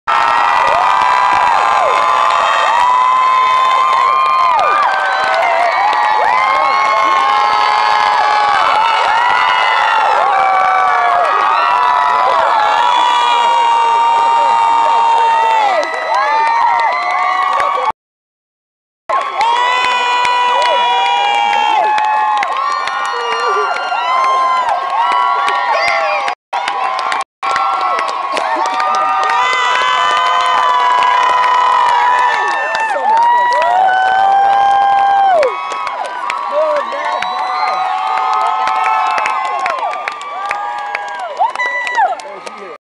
Extreme Makeover: Home Edition Grommesh family comes home
The Grommesh family comes home on Day 7 for the MOVE THAT BUS moment, as the crowd of thousands patiently waits in Moorhead.  This was part of the Extreme Makeover: Home Edition build that took place in Moorhead, MN, October 2010.